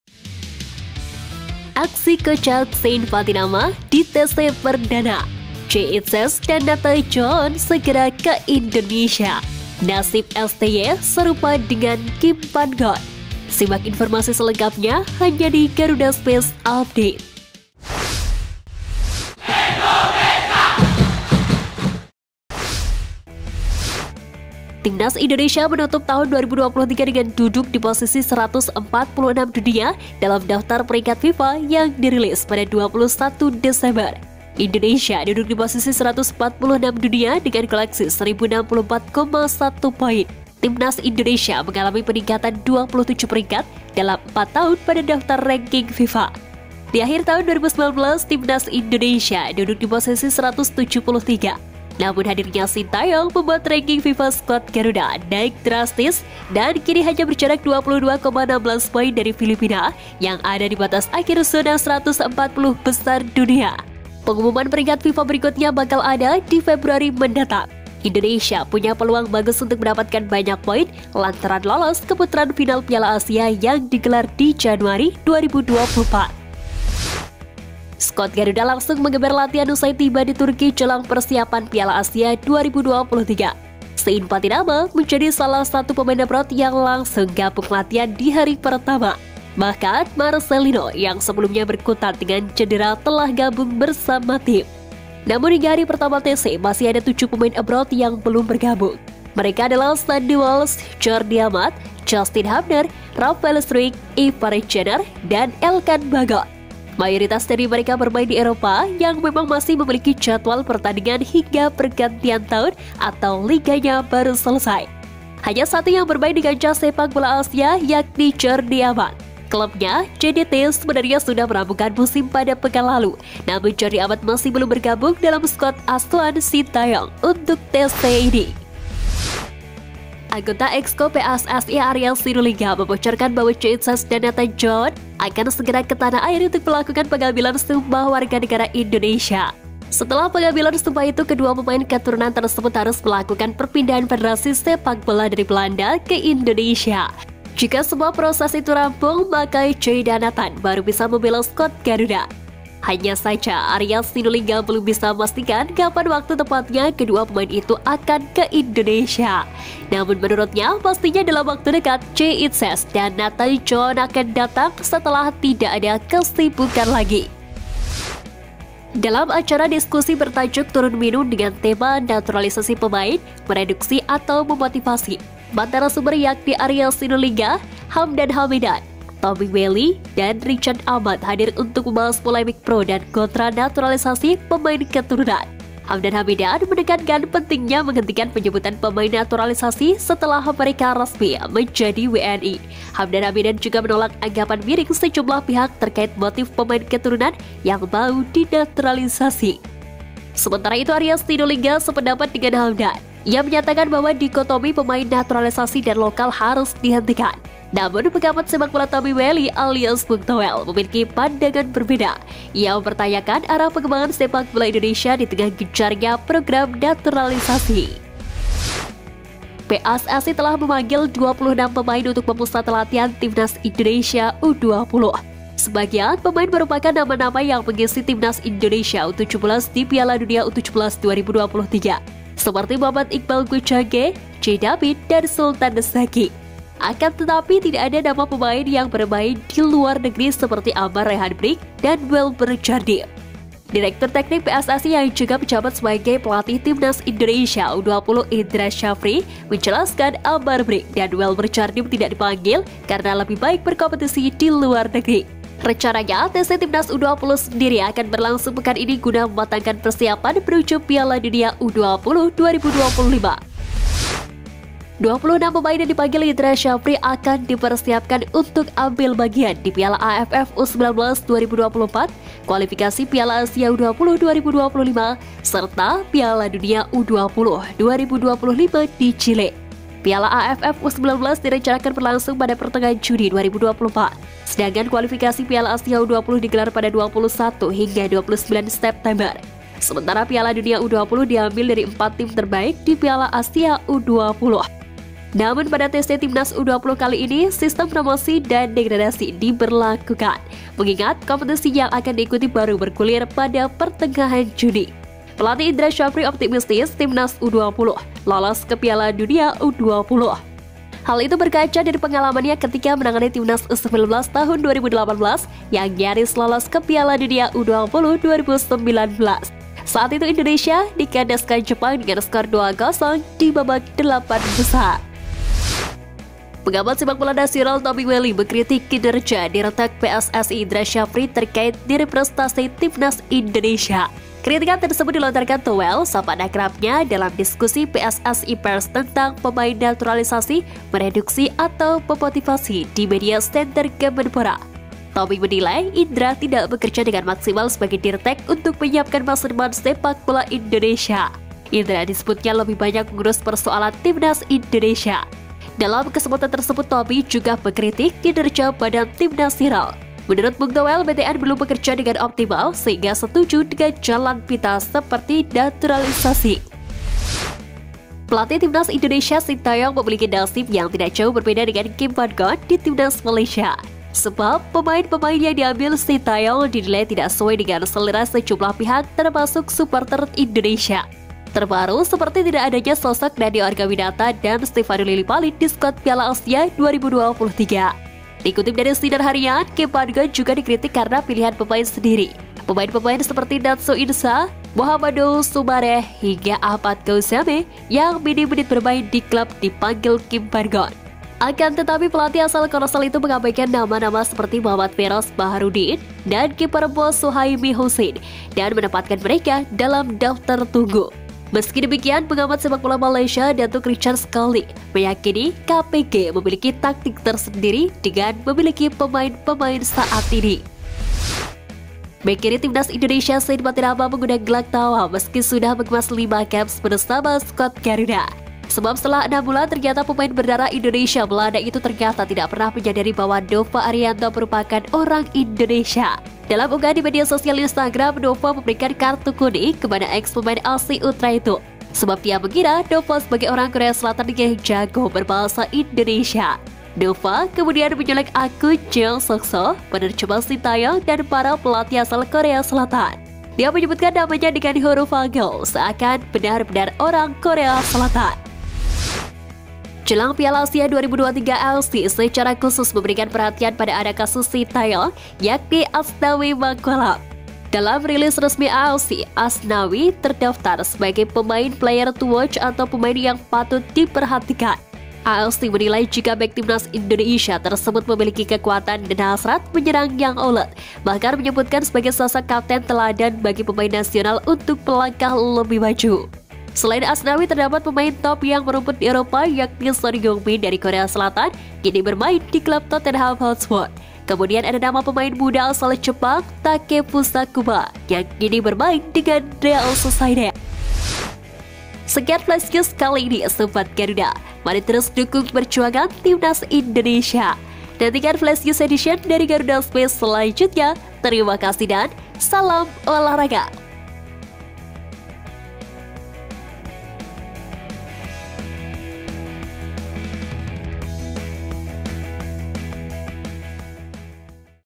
PATTYNAMA KOCAK DI TC PERDANA! Nasib STY Viral di Malaysia~Sumpah WNI Jay SEGERA~Timnas OPTIMIS
BERITA BOLA TERBARU DAN TERUPDATE DARI GARUDA SPACE - Berita Bola Hari Jum'at, 22 Desember 2023 : PATTYNAMA KOCAK DI TC PERDANA! Nasib STY Viral di Malaysia~Sumpah WNI Jay SEGERA~Timnas OPTIMIS